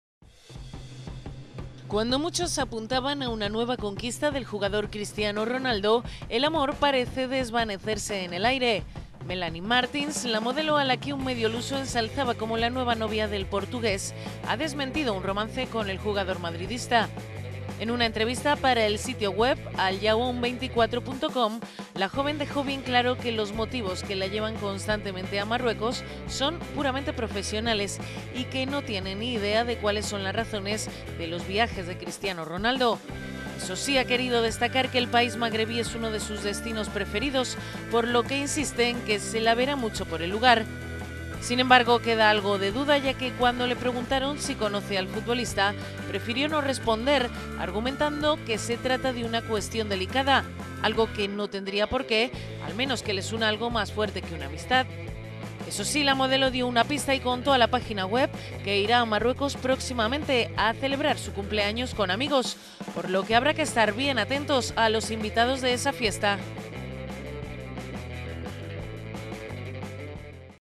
Melanie Martins desmiente relación con Ronaldo
Melanie Martins, la modelo a la que un medio luso ensalzaba como la nueva novia del portugués ha  desmentido un romance con el jugador madridista. En una entrevista para el sitio web alyaoum24.com, la joven dejó bien claro que los motivos que la llevan constantemente a Marruecos son puramente profesionales.